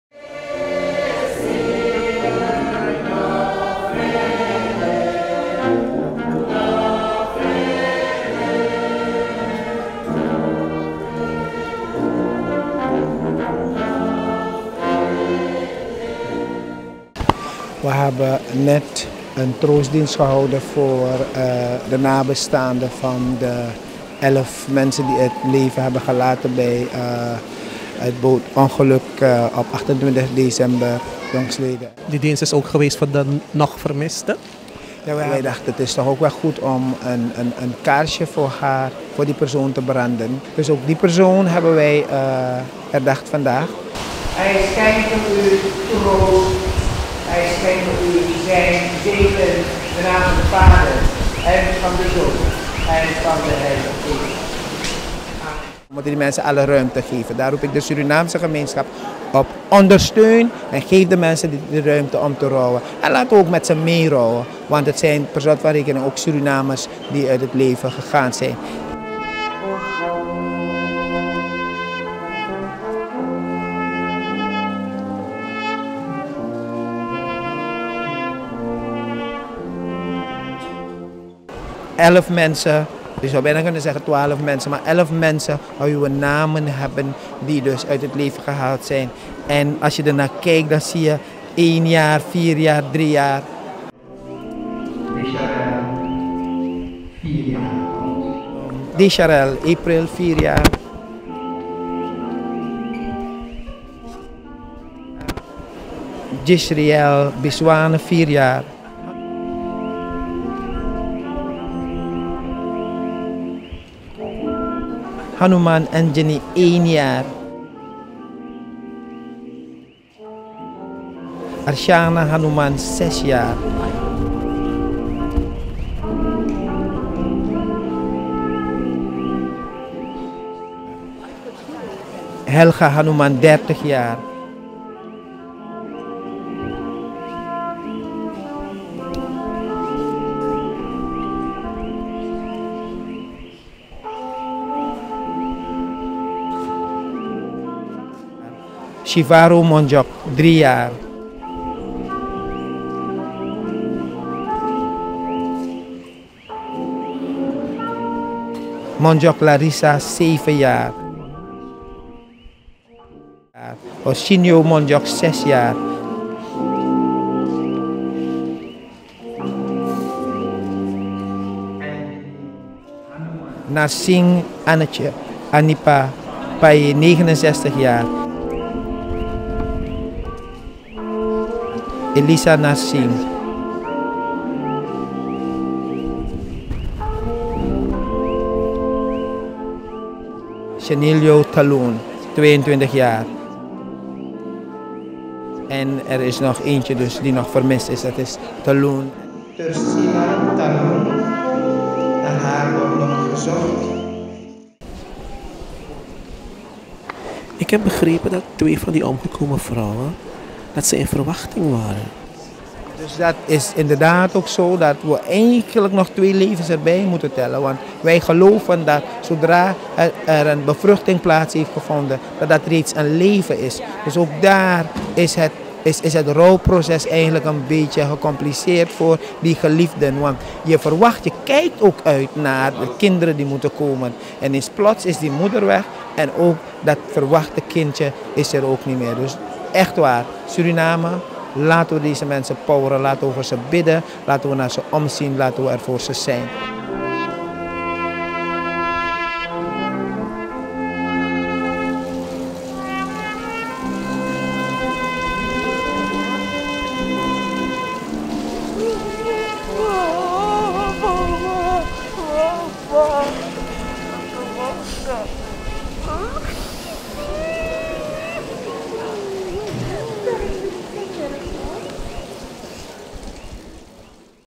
Rouwdienst voor drenkelingen Coppenamerivier
StarNieuws Suriname
In de Memre Buku kazerne zijn donderdagavond de slachtoffers van het bootongeluk van 28 december op de Coppenamerivier, met een rouwdienst herdacht. Op een tafel branden twaalf kaarsen: elf voor de vrouwen en kinderen wiens lichamen geborgen zijn en één voor degene die nog vermist wordt. Het verdriet is intens wanneer de namen en leeftijden van de overledenen worden genoemd.
Category
People & Blogs
License